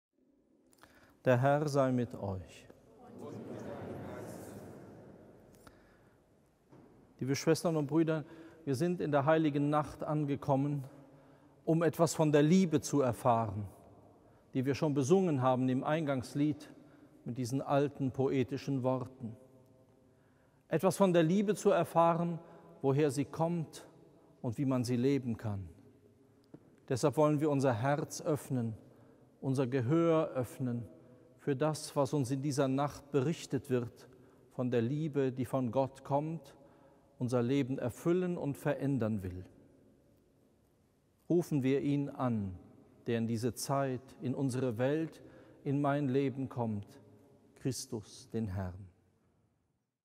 Eröffnungsworte zur Christmette 2015
Rektor Dr.Johannes J.Kreier im Kloster am rande de rStadt/ SB.